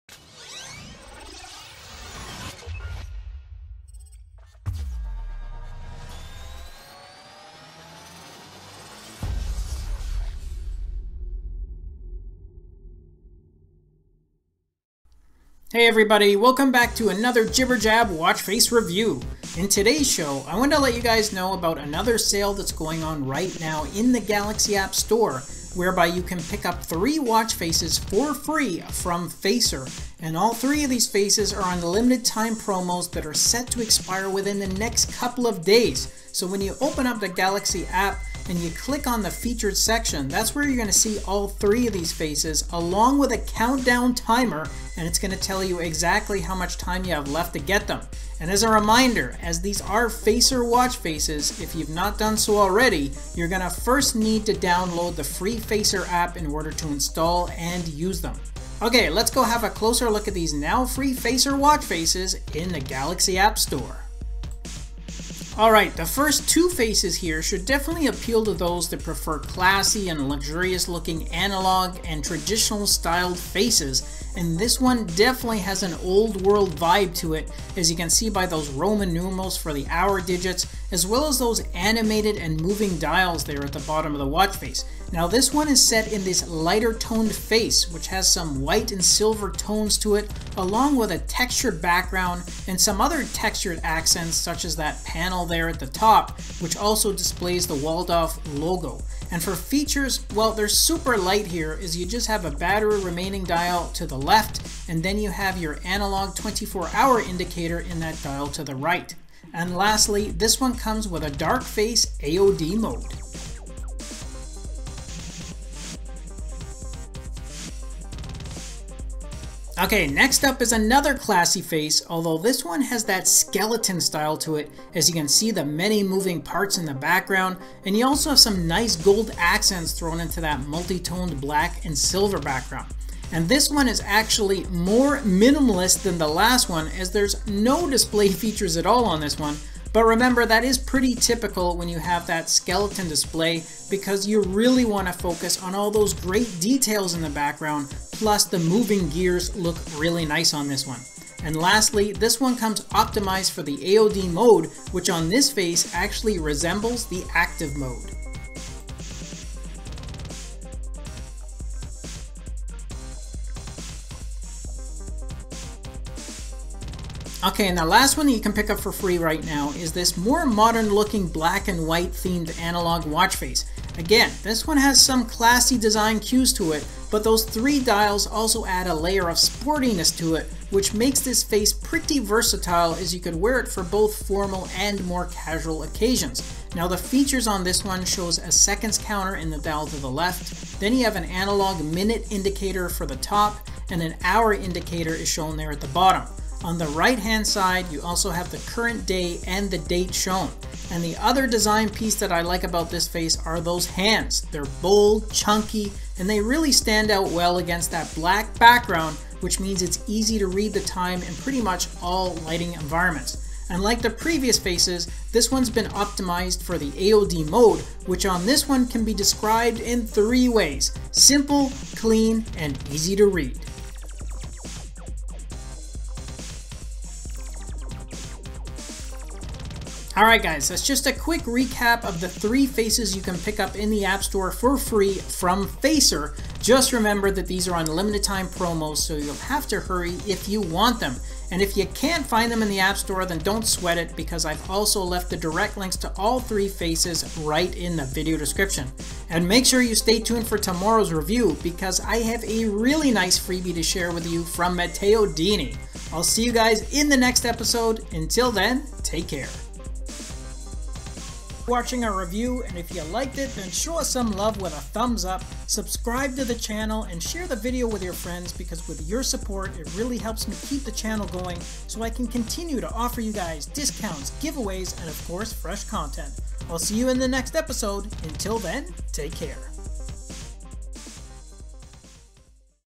*FREEBIE ALERT!* Samsung Galaxy Watch/Gear Watch Faces by Facer - Jibber Jab Reviews!
Pick up these 3 Facer watch faces in the Galaxy app store for free but only for the next couple of days!

Samsung Watchfaces
Smartwatch